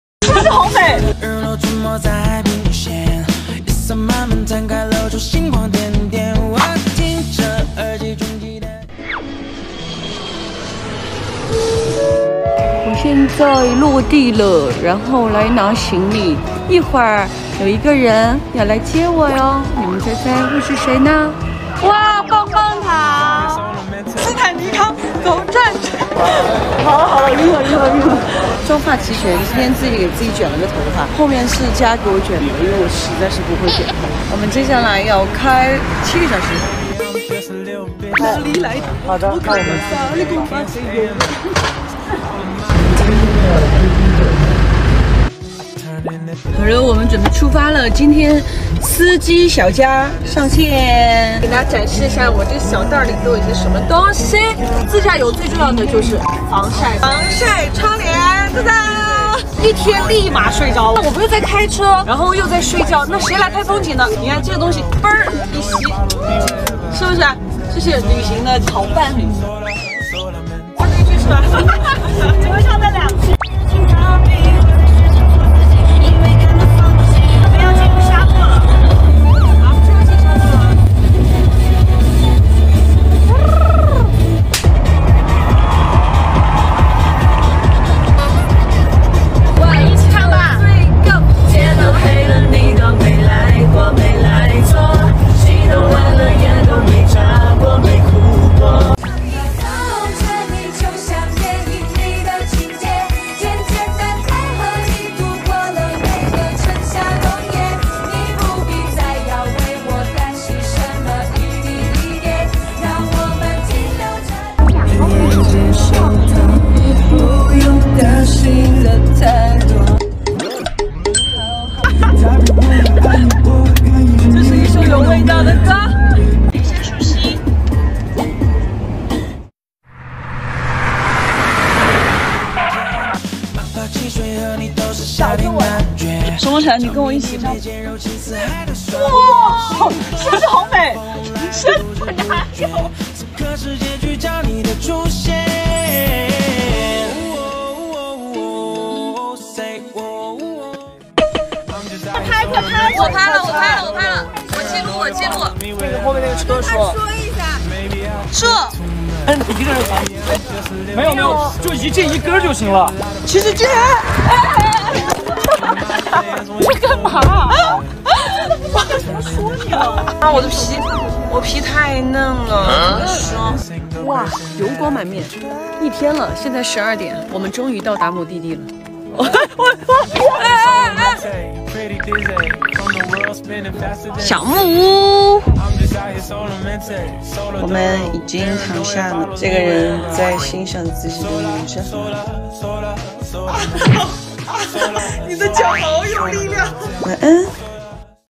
孟佳 in 沈梦辰 新疆之旅vlog 1 20220729
你们期待已久的新疆之旅第一期，来咯🤗 怎么能不期待和好朋友的旅行呢？@M孟佳J
尽兴生活

Cr：沈梦辰微博